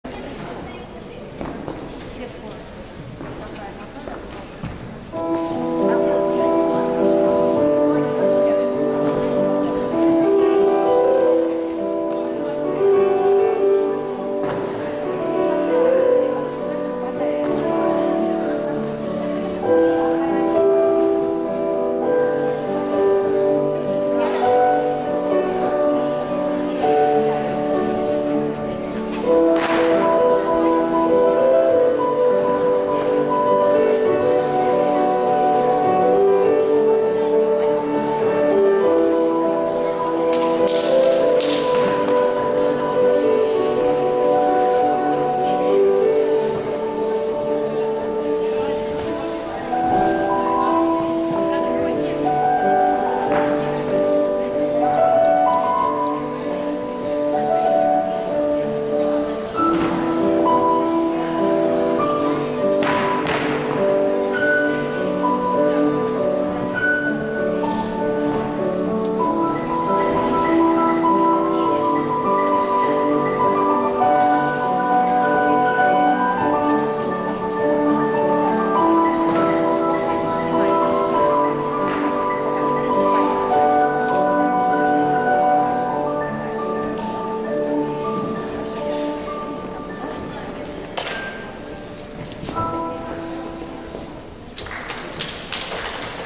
Gagarin Cup, Космонавты-2011 Художественная гимнастика 10
Зал Крылья Советов, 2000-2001г. р. Второй день, 3 и 4 виды. Художественная гимнастика Rhythmic Gymnastics